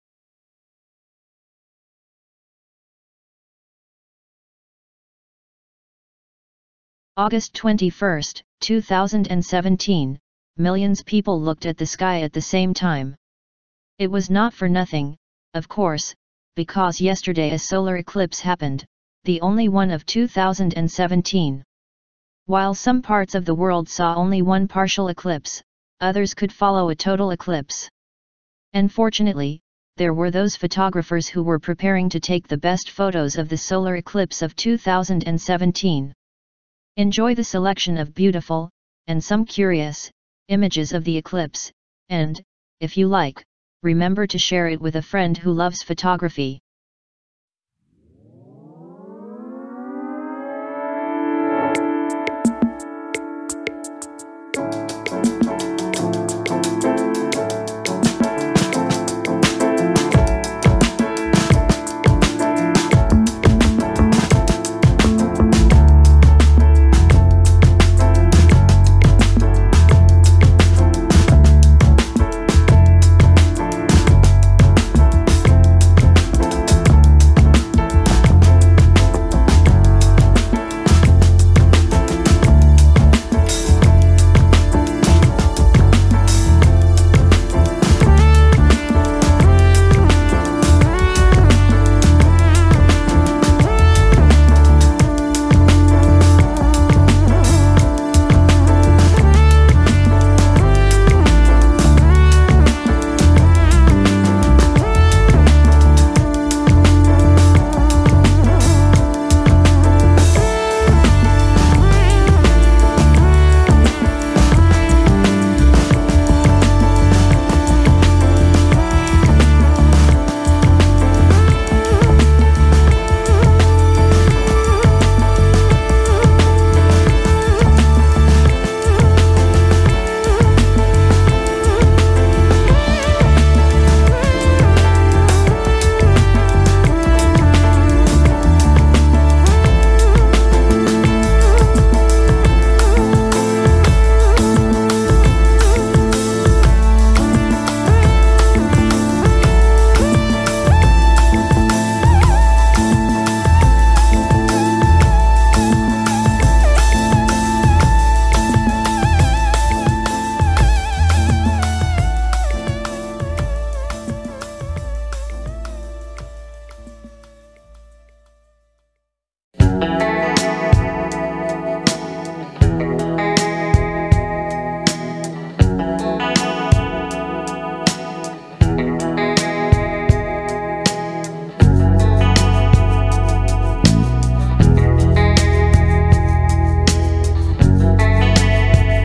Solar Eclipse of August 21, 2017!!! The 22 Best Photos of Eclipse
August 21, 2017, millions people looked at the sky at the same time.